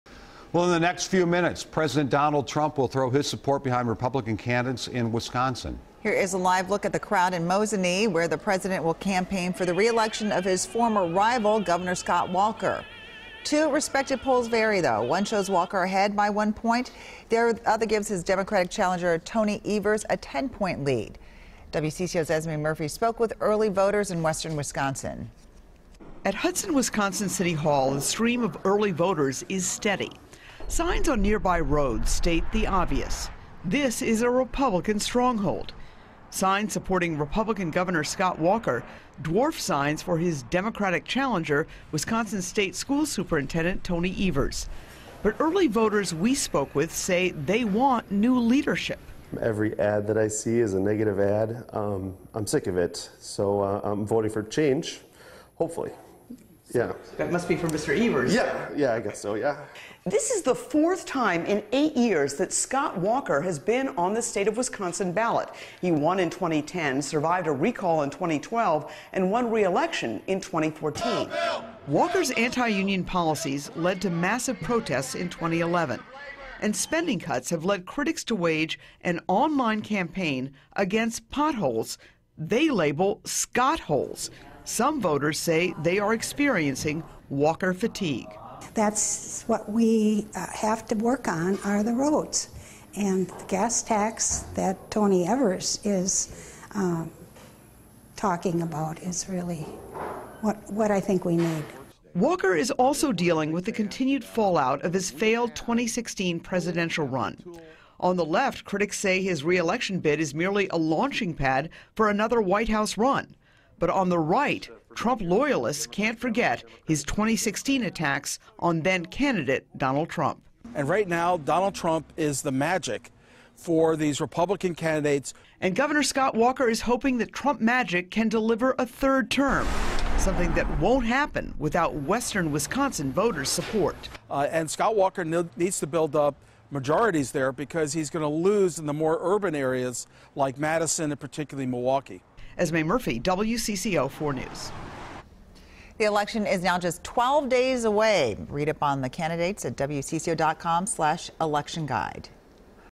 Trump Campaigning For Scott Walker In Rural Wisconsin
President Donald Trump is in Mosinee, Wisconsin Wednesday, campaigning for Scott Walker and Leah Vukmir, Esme Murphy reports (2:32). WCCO 4 News at 6 - Oct. 24, 2018